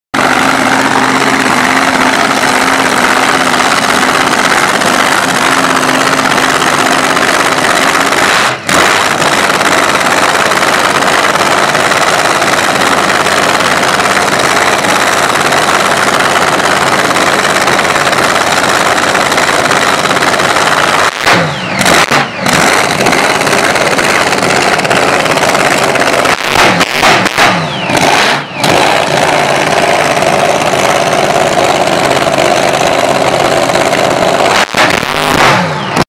loudest Dragster engine ever hurts the ears.
Dragster demonstrates engine at car show,the sound is so loud it hurts the ears.